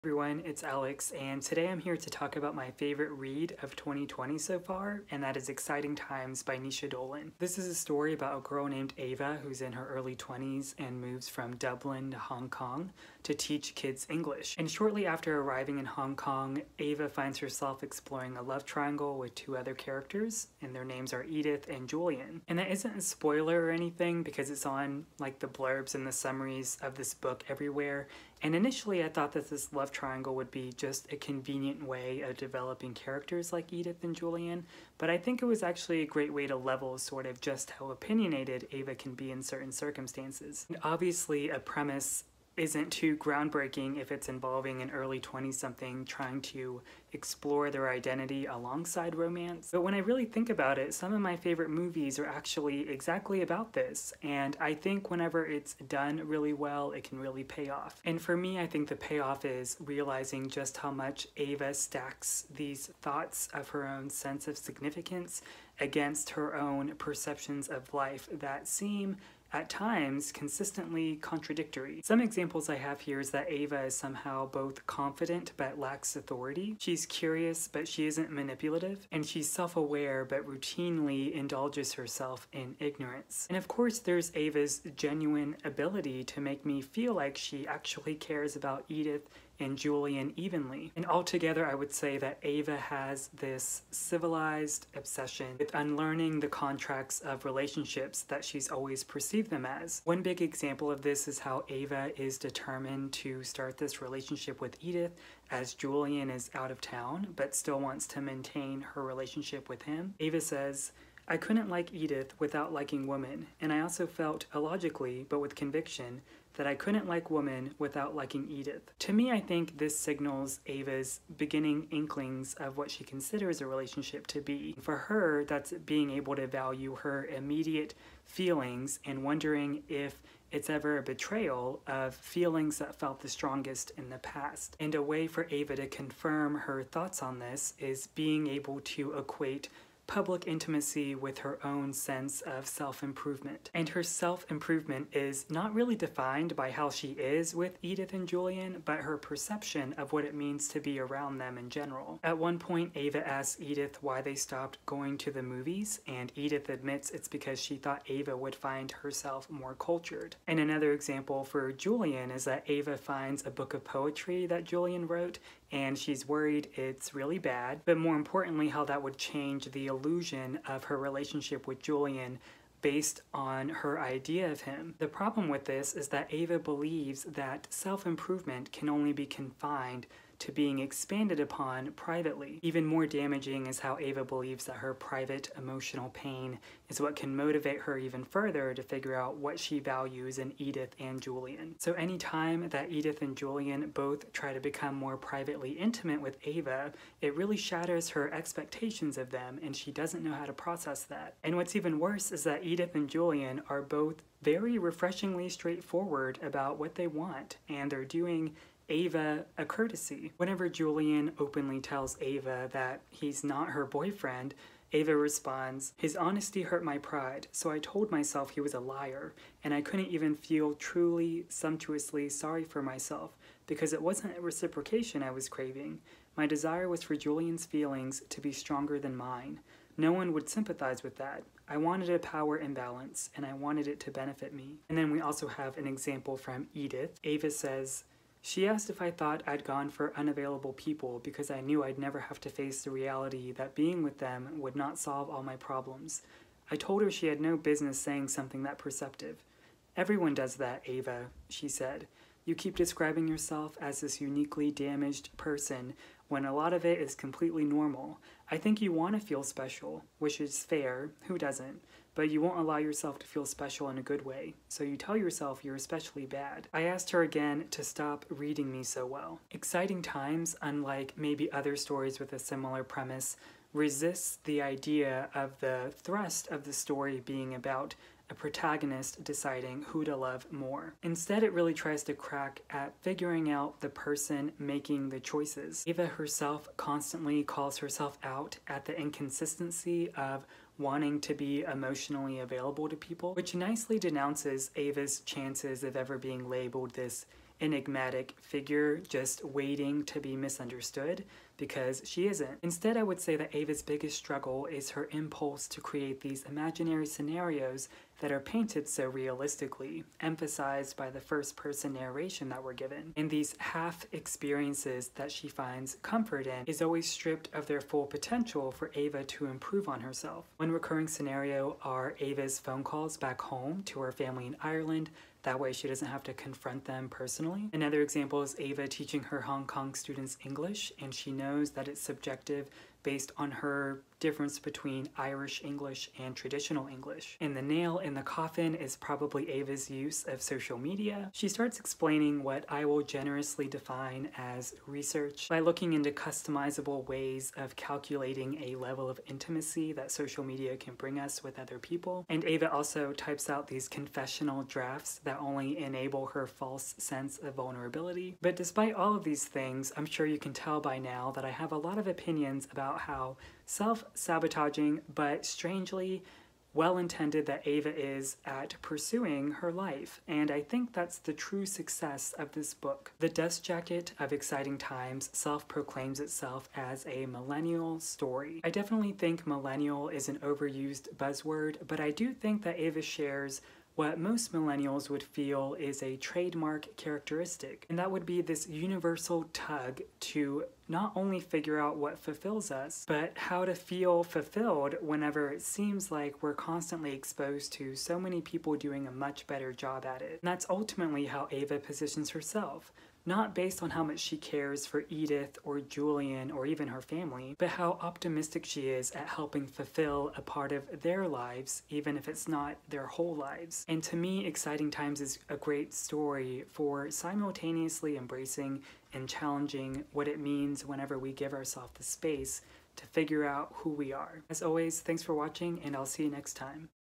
Exciting Times | Book Review
Think The Parent Trap but it’s Naoise Dolan and Sally Rooney and their parents are Sofia Coppola and Wong Kar-wai